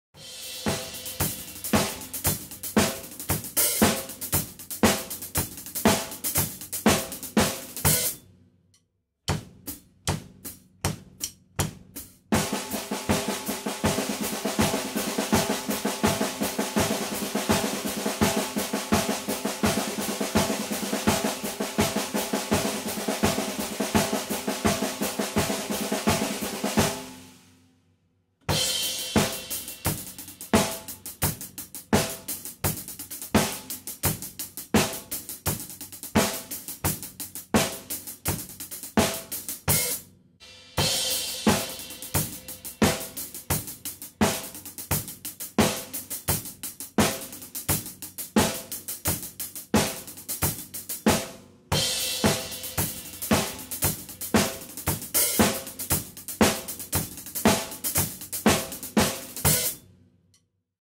ドラム練習！今日のプチレッスン！【No.650】シックスストロークロールをさりげなく使う方法！
今日のプチレッスン！【No.650】
Level.★★★☆☆
本日はシックスストロークロールをさりげなく使う方法！16分の動きの中で6連符をさりげなく入れれると16ビートの時などの表現が豊かになります！

『スタジオラグへおこしやす』のドラム教則関連記事のライターも勤めてます。